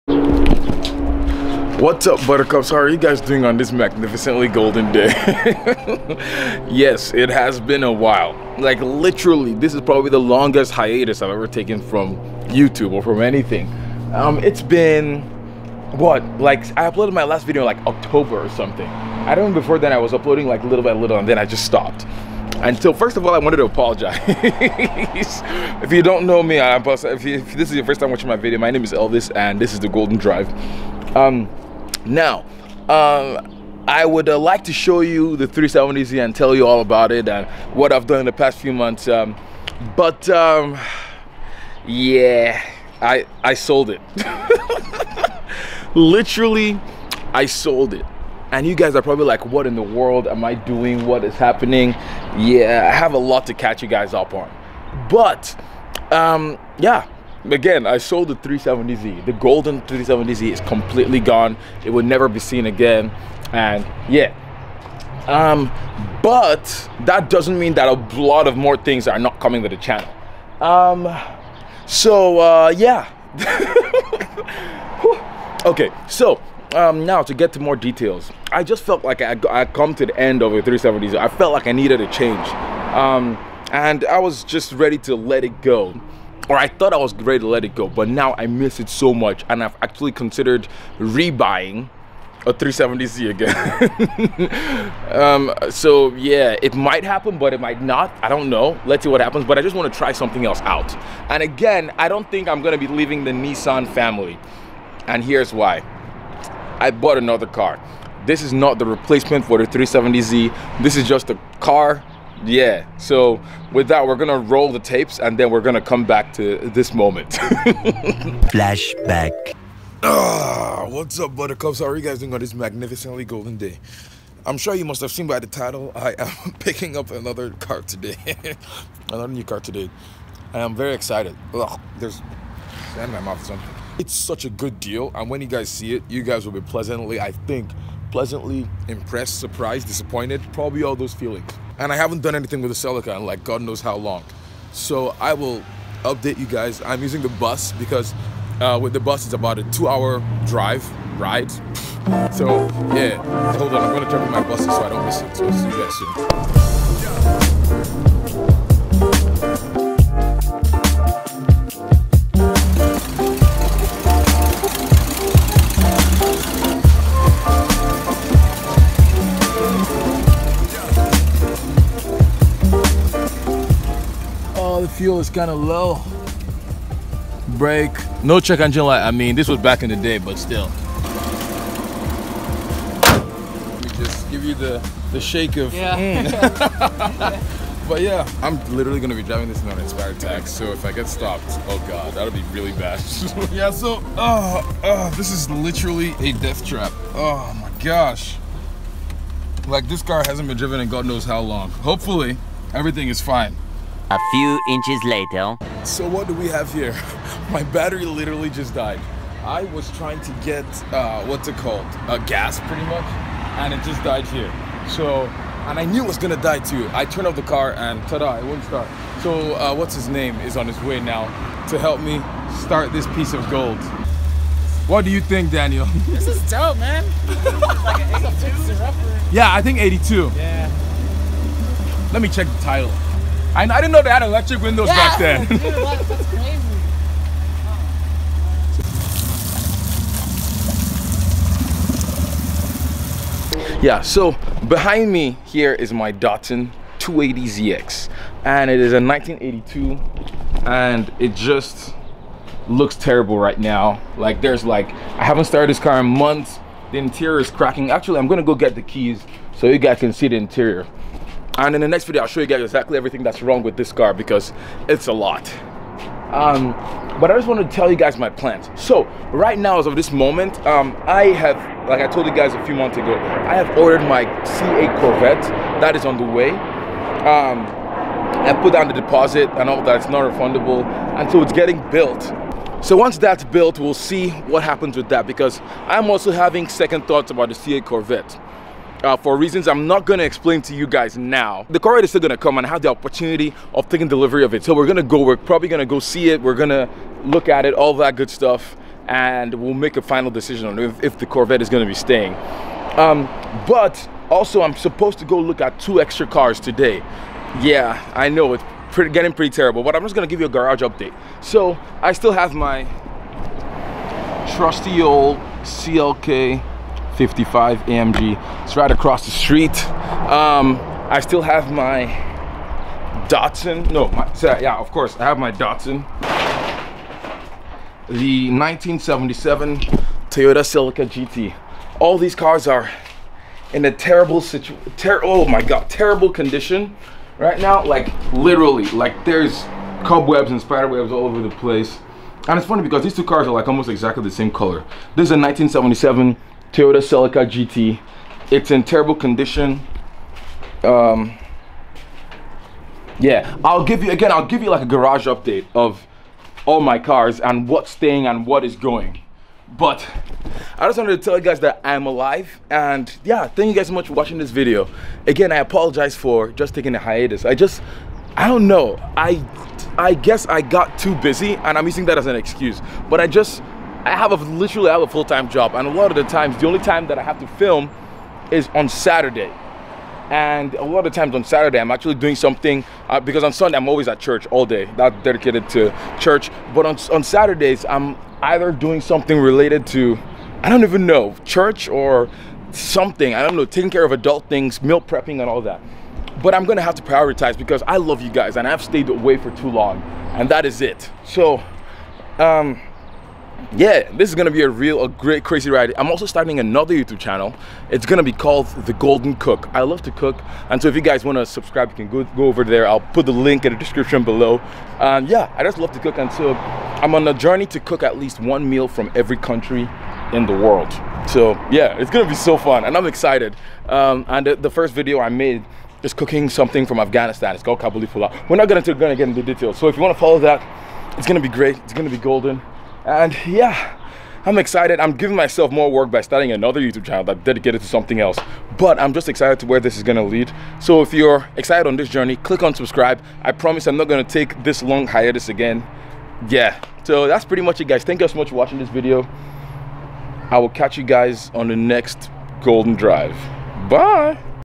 I SOLD my Nissan 370z and Got a NEW Car
So... as the title of the video says, I sold my Nissan 370Z which I've had for almost 4 years and got another car but it's not what you're thinking! I took a really long Hiatus from Youtube but I'm back and I'm so excited!